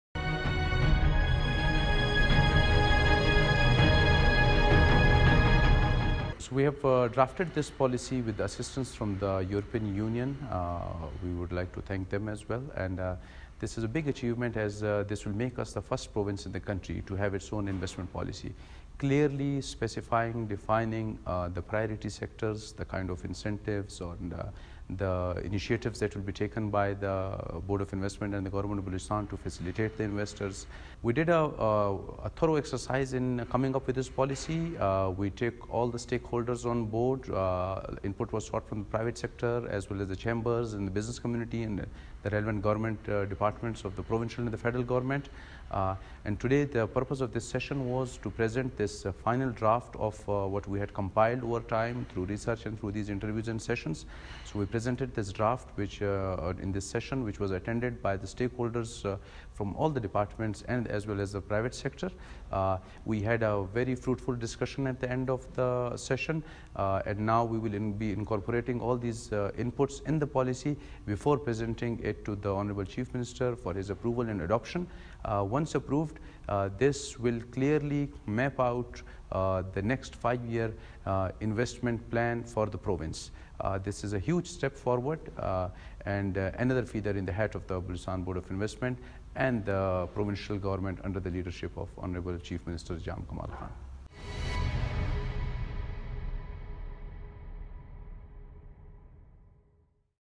Mr. Zarkoon speaking at stakeholders' consultative session on Balochistan’s First Investment Policy
CEO BBOIT, Mr. Farman Zarkoon, speaking passionately at the consultative session of the stakeholders on Balochistan’s First-ever Investment Policy.